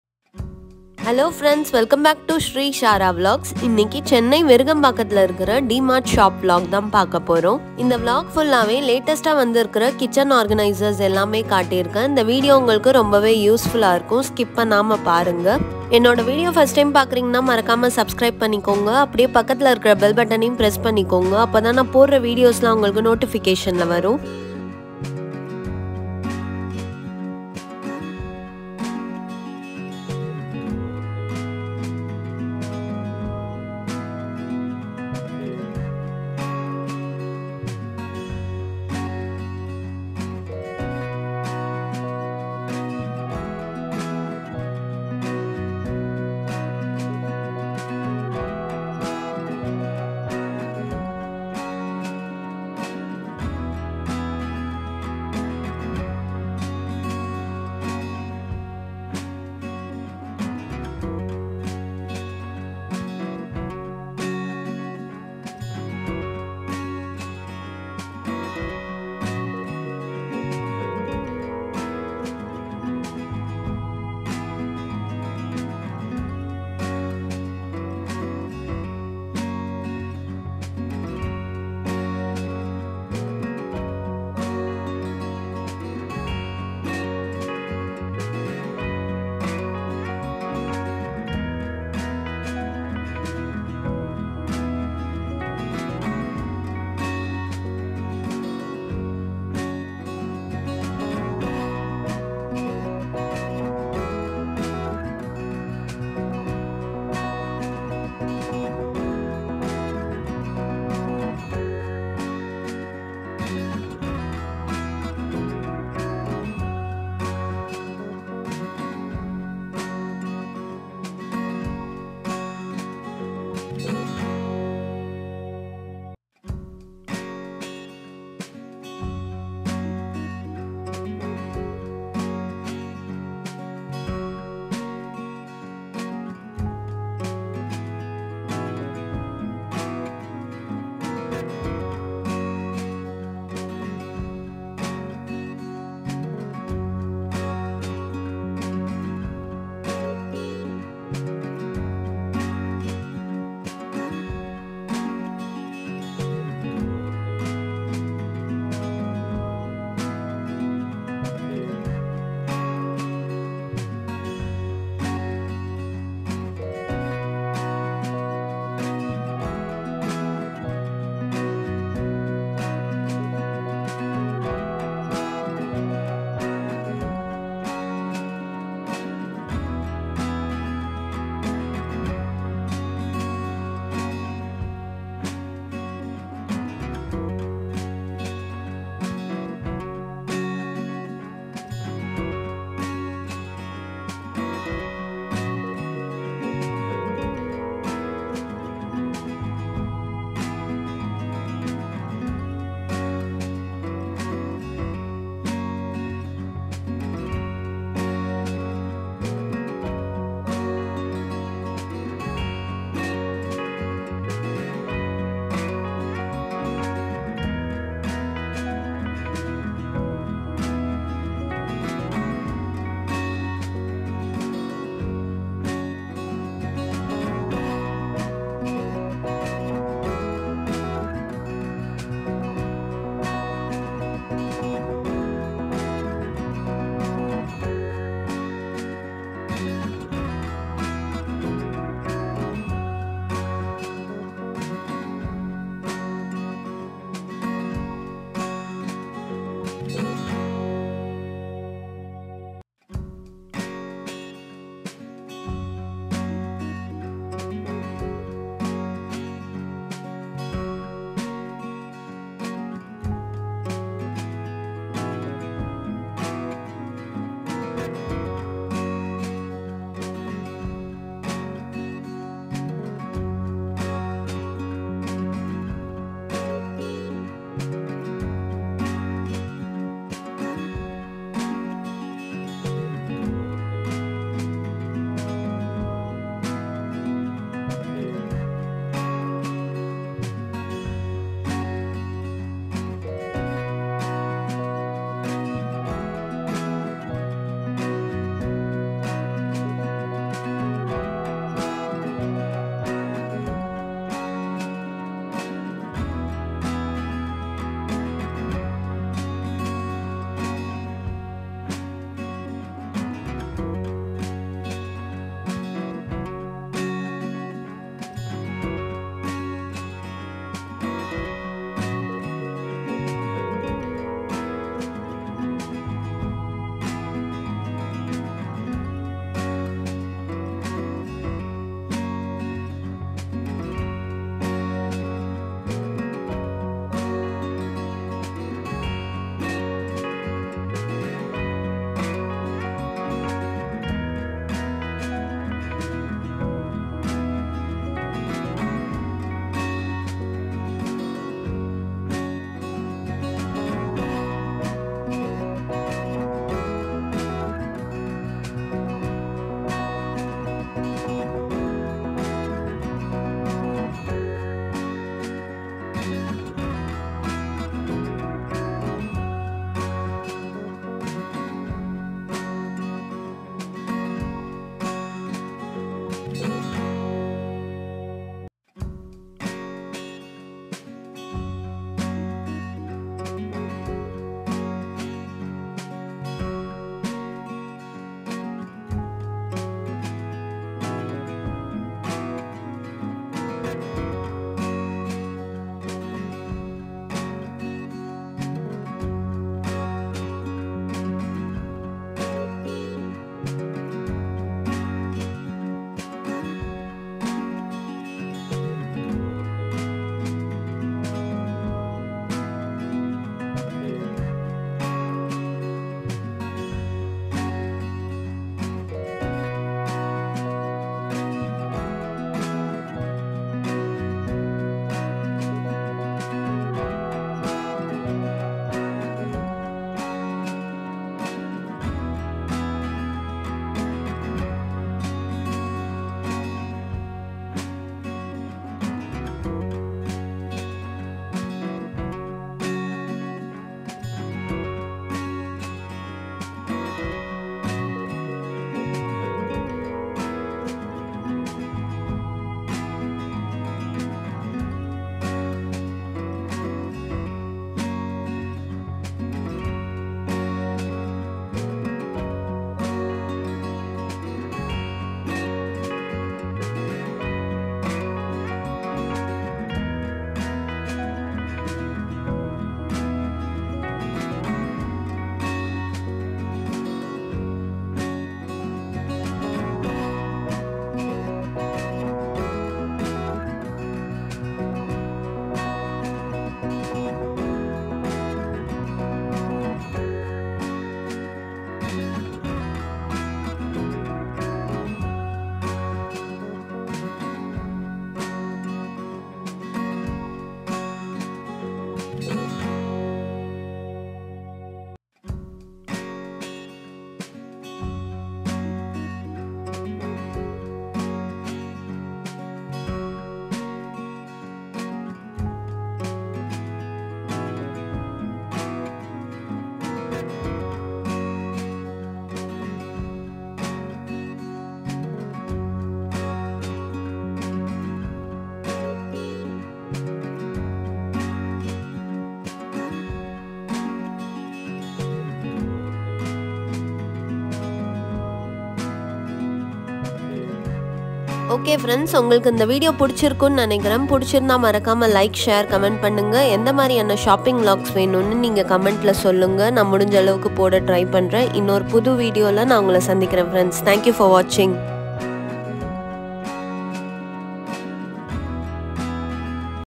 D Mart Cheap Kitchen Products | Latest Stainless Steel Collections | Latest Offers | New Arrivals
This vlog is about D MART VIRUGAMBAKKAM, Chennai. I hope you will enjoy my video.

Shop Address :
D MART
Chandra Metro Mall, Arcot Road, Rajeswari Colony
Virugambakkam, Chennai-600092